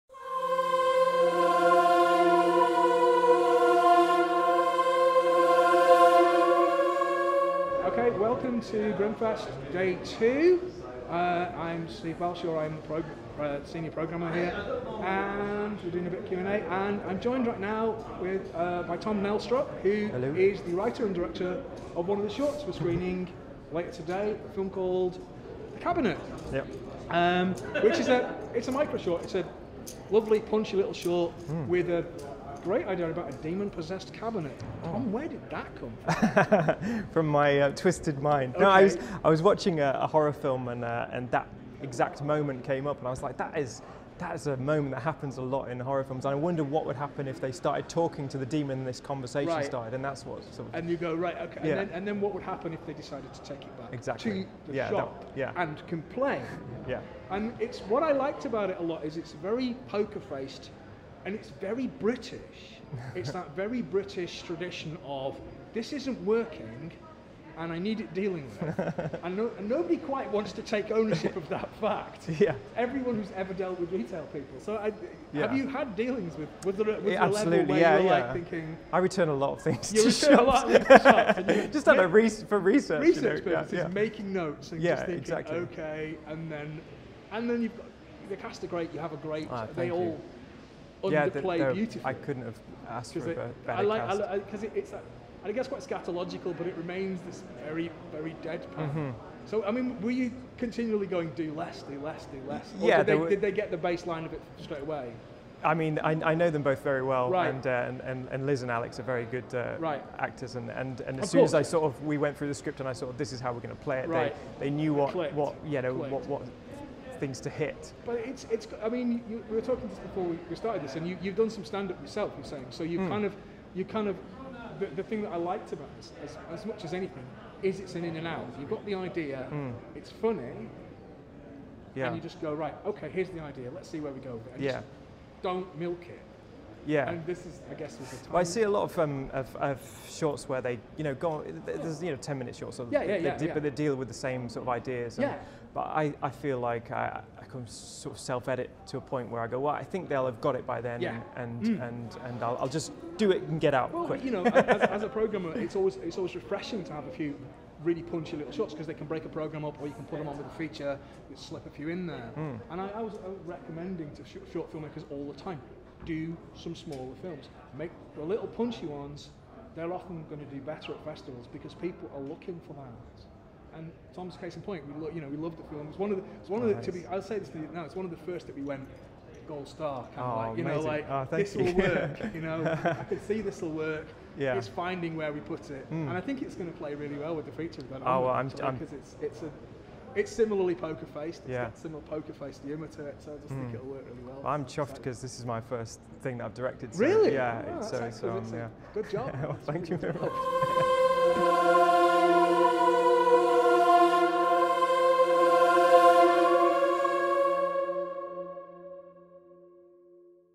Grimmfest 2019 || 360 Interview With Tom Nelstrop || + BTS
Steve Balshaw interviews Tom Nelstrop, Director of ' The Cabinet ' Shown at Grimmfest this year.

Special thanks to the Grimmfest team. Paul Sparkes and Michael Booth for the audio.